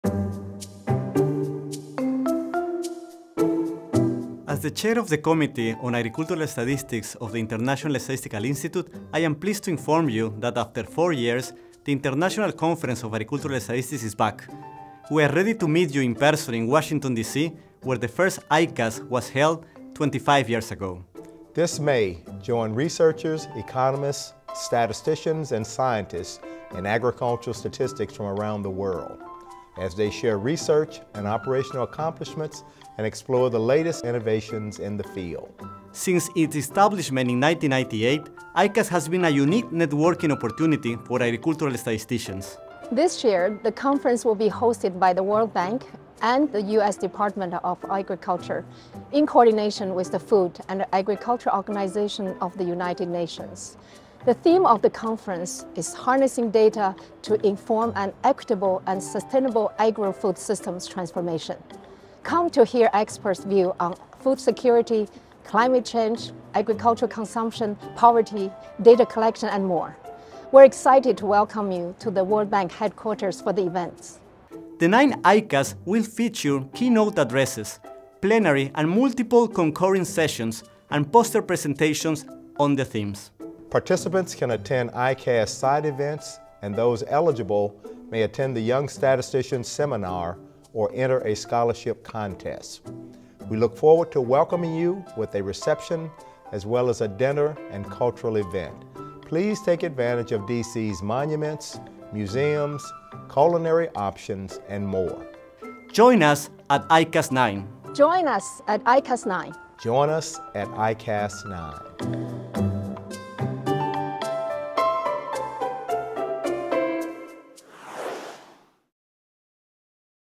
The Ninth International Conference on Agricultural Statistics
The Ninth International Conference on Agricultural Statistics (ICAS IX) will be held from May 17 to 19, 2023 in Washington, DC, and will be organized by the @UsdaGov and the @WorldBank in coordination with the @UNFAO . The Theme of the conference is Harnessing Data to Inform an Equitable and Sustainable Agri-Food Systems Transformation. Register today and join us at ICAS IX!